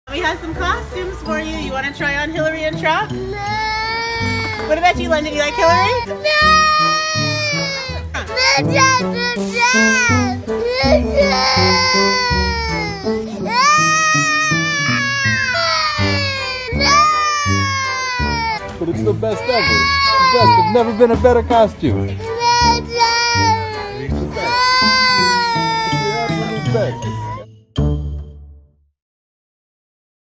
Trump mask sends child into a frenzy
Donald Trump's campaign slogan is that he wants to "Make America Great Again," but 2-year-old Miles isn't having any of it. On what's been described to The Post as a routine Walmart shopping spree after attending church, a mother wanted to take a selfie with her kids in pretend Halloween costumes, opted to wear a Trump mask, and, well, her son Miles' reaction is priceless.